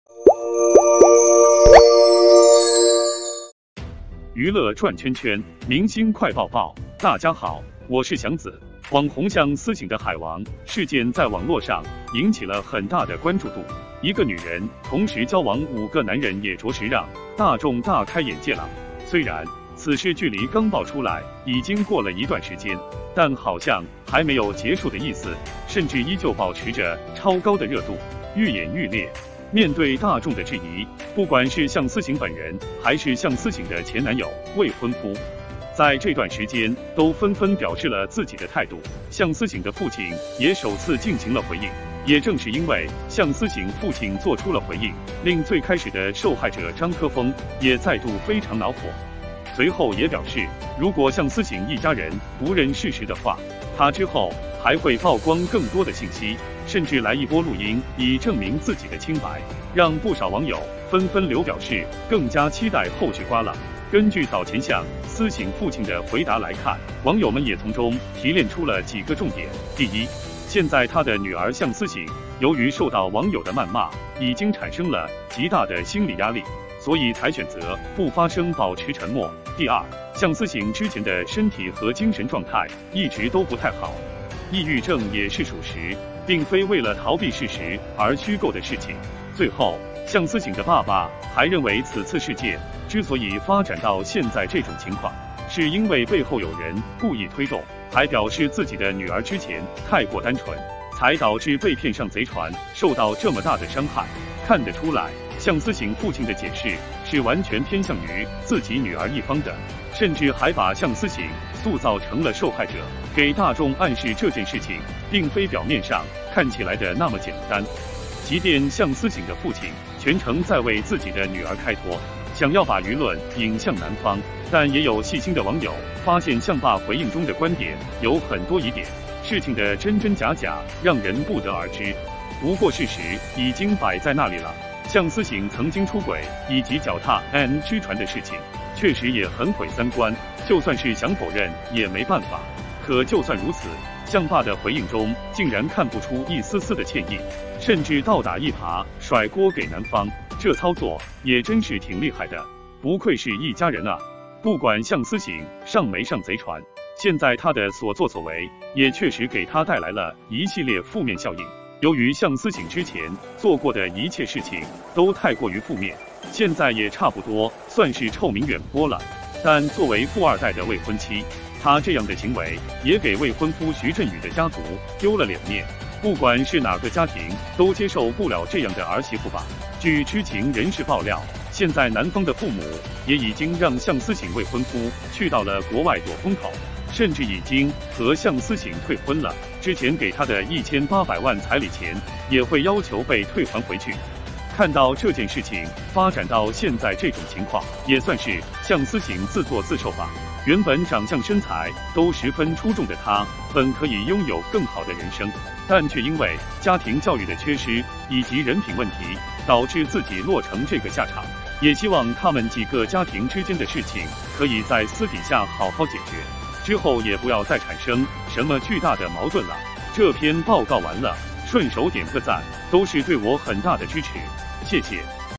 项思醒的前男友怀疑他会曝光录音材料，张科峰对父亲的回应很恼火
网红项思醒的海王事件在网络上引起了很大的关注度，一个女人同时交往5个男人也着实让大众大开眼界了。虽然，此事距离刚爆出来已经过了一段时间，但好像还没有结束的意思，甚至依旧保持着超高的热度，愈演愈烈。